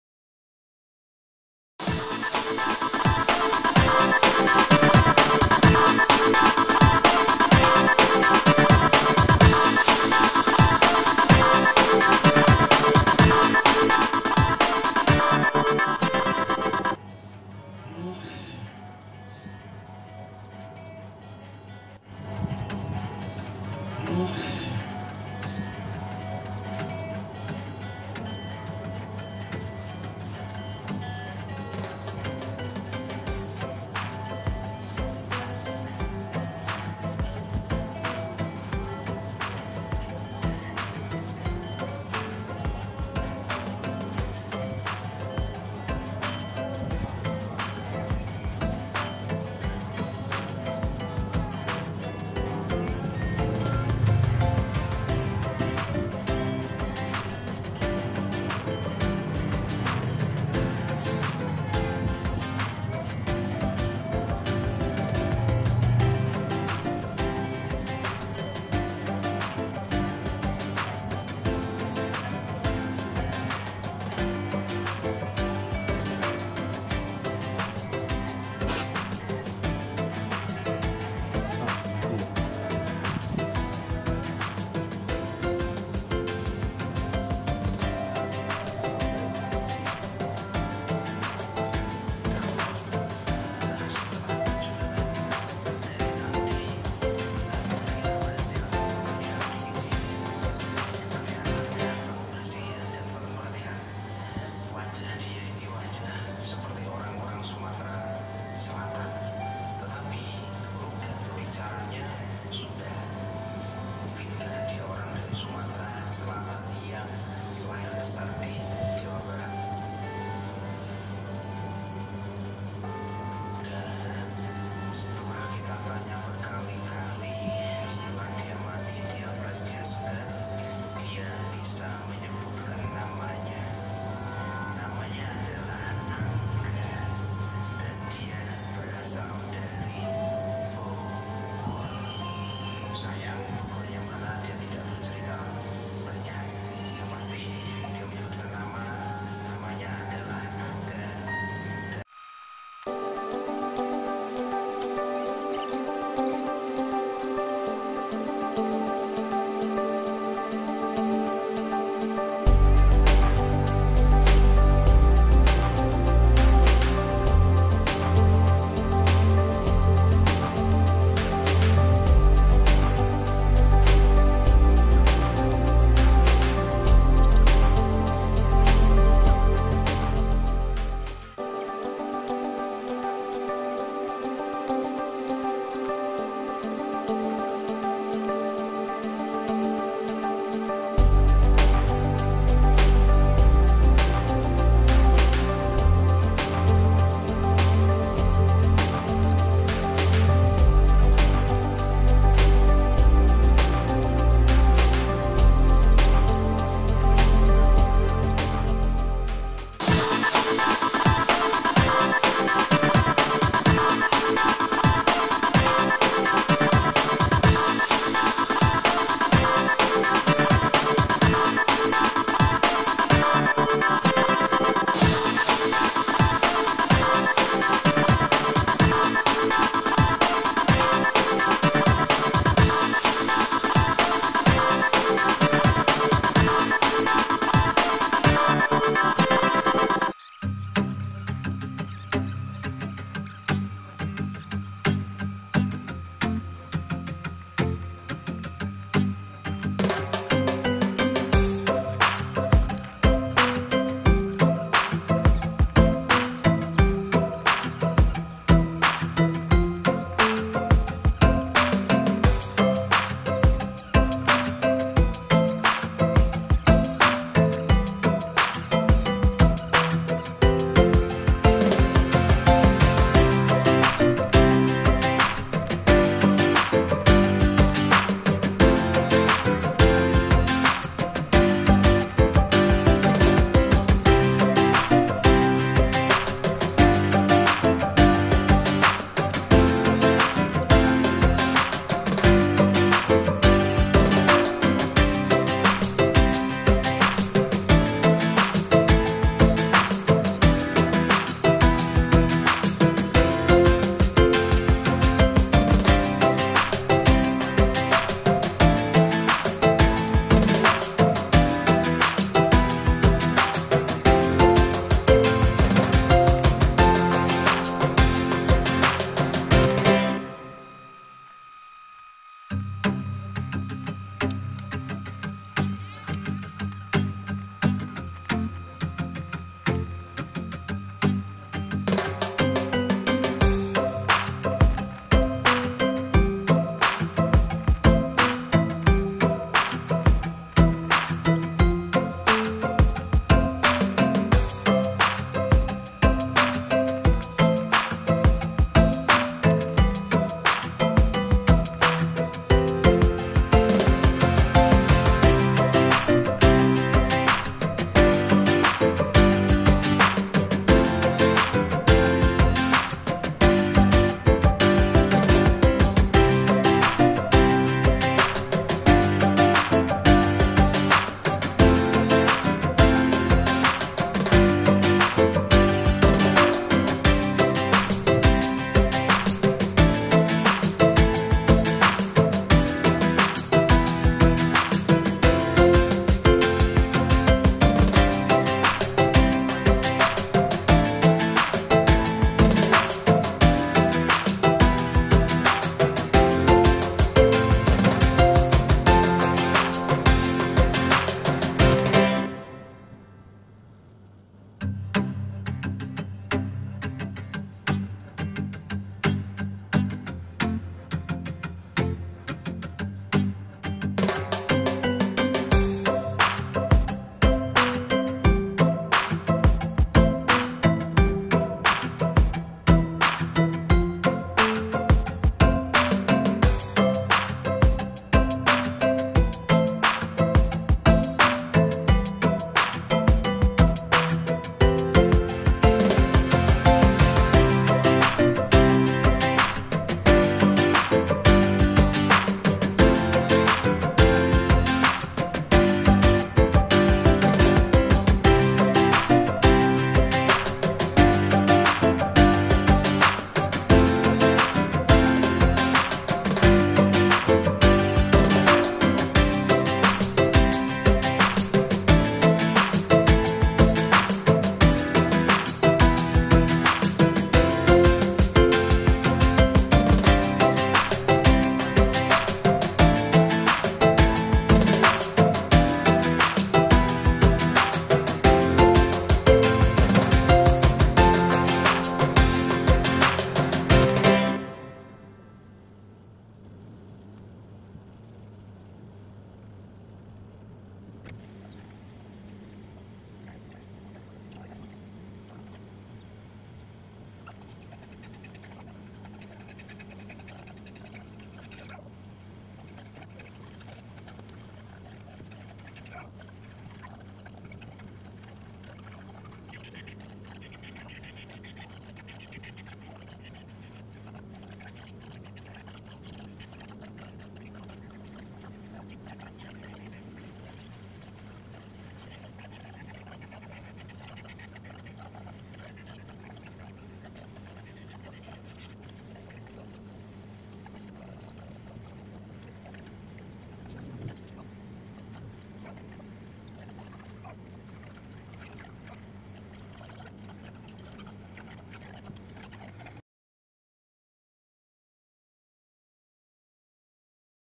Cara melukis/lukisan pemandangan alam (akrilic painting)
Alam indah dengan air terjun dan sungai yang mengalir